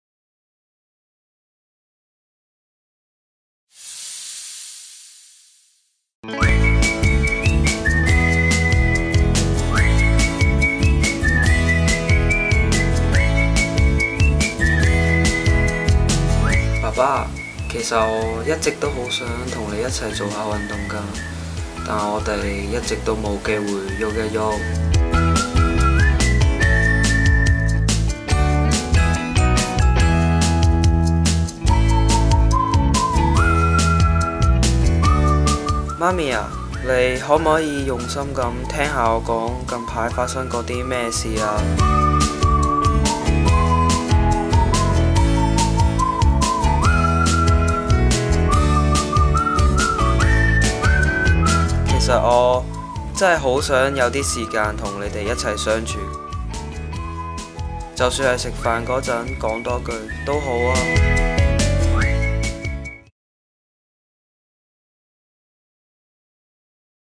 聽．沙聲 Painting my sound
UNICEF HK「零，可以成真」短片項目2014
方永傑（香港青少年服務處）

故事大綱：現今社會，人人都為生活而努力工作，希望讓生活質素提高。漸漸家庭也出現了問題。父母忙於自己的工作，辛苦一天後，回家只會向自己的兒女訴苦工作的疲累。而又忘記了和自己兒女一起玩樂、聊天的時間。

UNICEF HK Believe In Zero, Make A Video 2014
FONG Wing-kit (Hong Kong Children & Youth Services)

Synopsis: Nowadays, people work hard to lift living standard. Gradually, there is family issue. After a busy day, parents grumble about their tiredness while not spending time to play with or talk to their children.